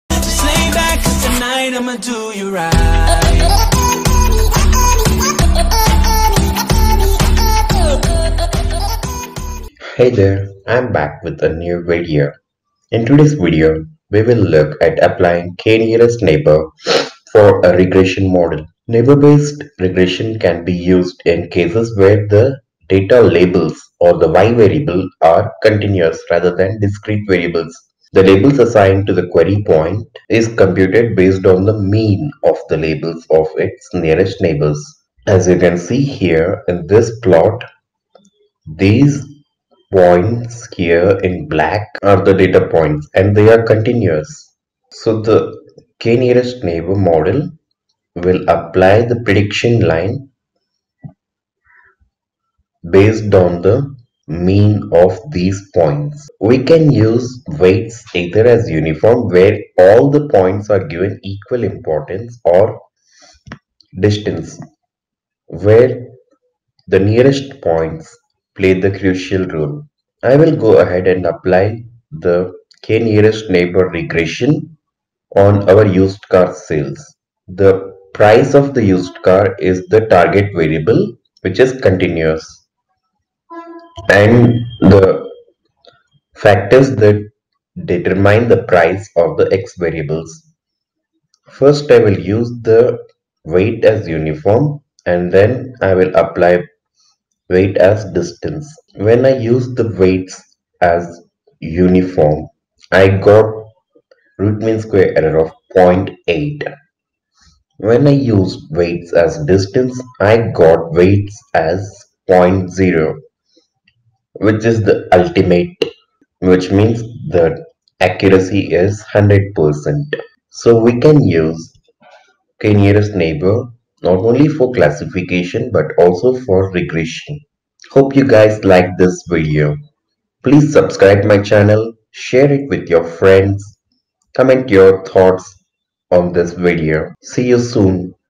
Day 17 of 100 Days to ML | K Nearest Neighbor Regression
In this video, we will learn how to apply Nearest Neighbor algorithm to Regression model.